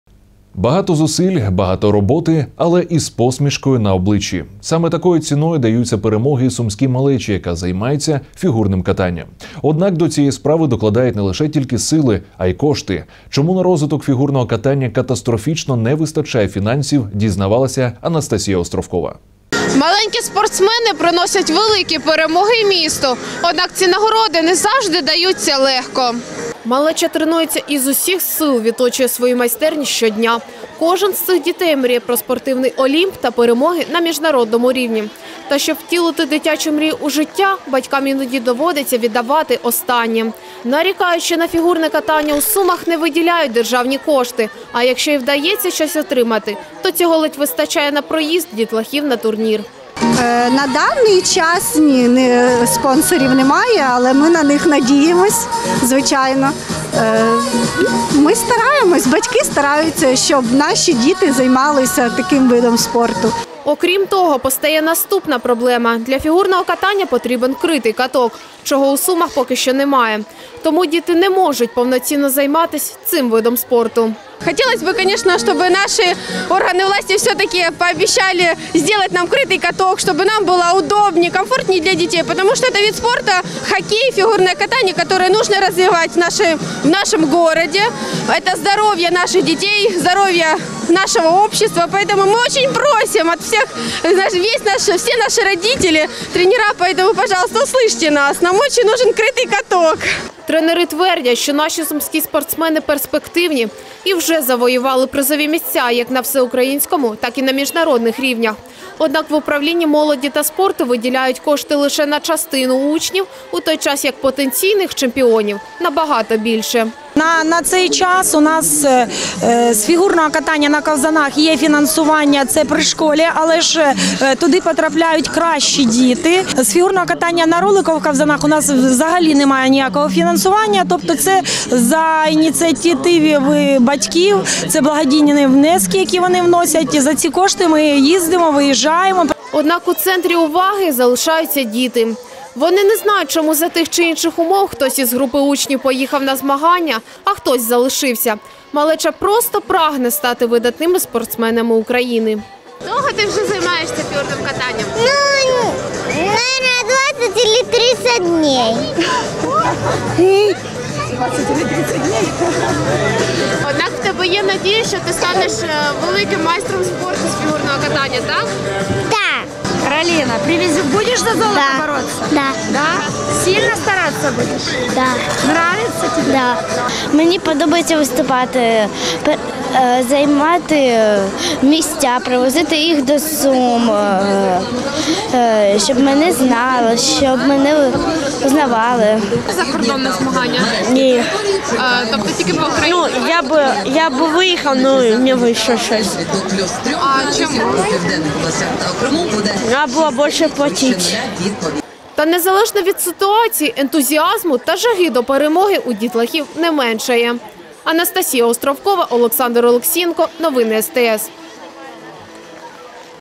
Маленькі фігуристи мріють про перемоги. Коштів на це не вистачає
Багато зусиль, багато роботи, але із посмішкою на обличчі – саме такою ціною даються перемоги сумській малечі, яка займається фігурним катанням. Однак до цієї справи докладають не тільки сили, а й кошти. Чому на розвиток фігурного катання катастрофічно не вистачає фінансів - дізнавалась Анастасія Островкова.